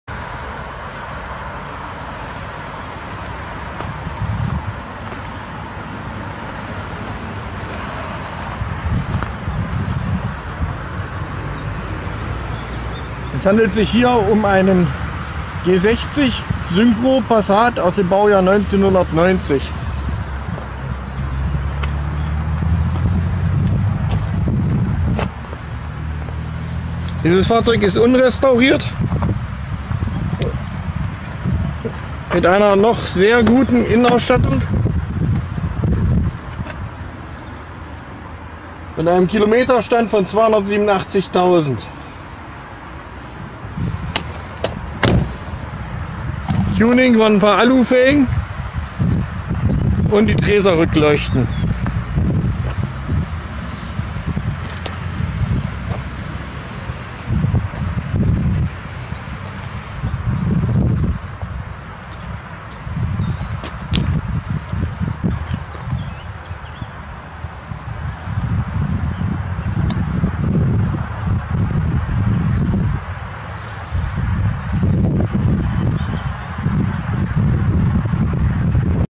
VW Passat Kombi G60 Scheunenfund Kloster Lorch
Auf dieser Tour habe ich einen VW Passat G60 Syncro aus dem Baujahr 1990 nach Wernigerode geholt. Der Passat ist in einem guten Zustand und wird mit einem H-Kennzeichen gefahren. Der Motor und der G-Lader laufen schön leise und werden noch einige Kilometer abspulen.
Im Hintergrund ist das Kloster Lorch zu sehen.